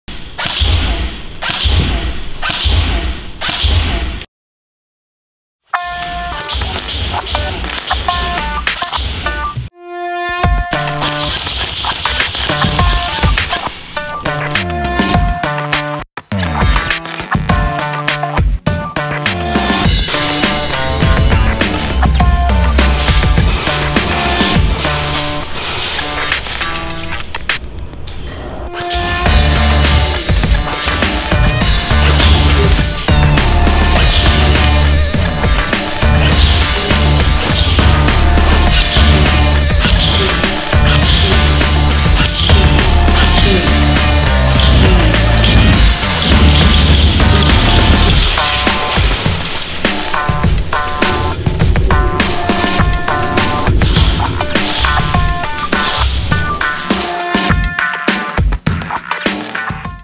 Target Practice on the shooting ranges at Blackout Airsoft!
Target practice at Blackout Airsoft on the Arcada interactive Airsoft Shooting range and steel plate range at Blackout Airsoft Experience Center!